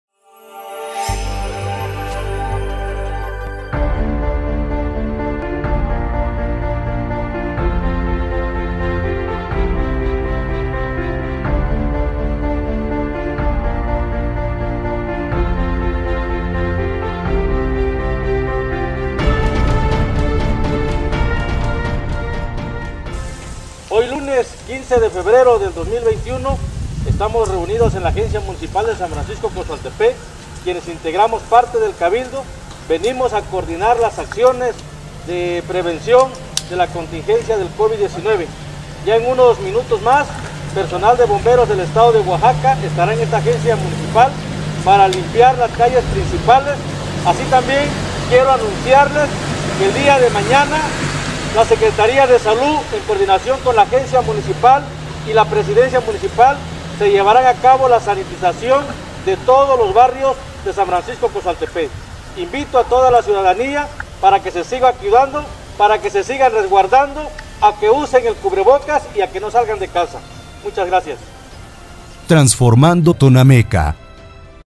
Gobierno municipal de Tonameca desinfecta calles de Cozoaltepec
Hugo Castrejón Martínez presidente Municipal de Santa María Tonameca informa:

Esta tarde en coordinación el Agente Municipal de San Francisco Cozoaltepec, Moisés Monajaras López y el Heroico Cuerpo de Bomberos de Oaxaca llevamos a cabo la desinfección de las principales calles de San Francisco Cozoaltepec, debido a que en esta comunidad en los últimos días se dió un brote de muchos casos positivos a la COVID-19.

El día de mañana, con el personal de vectores llevaremos una sanitización en todos los barrios.  Seguiremos trabajando a marchas forzadas para mitigar los contagios de este virus que ha cobrado vidas en nuestro municipio.  Agradezco a la Secretaría de Salud Oaxaca,  Jurisdicción Sanitaria Costa 04 y al Heroico Cuerpo de Bomberos GobOaxaca por el apoyo brindado.

Exhorto a la ciudadanía en general a usar el cubrebocas,  mantener la sana distanciaa y aplicarse gel antibacterial.  Cuidemonos por el bien de todas y todos.

Transformando Tonameca Oaxaca.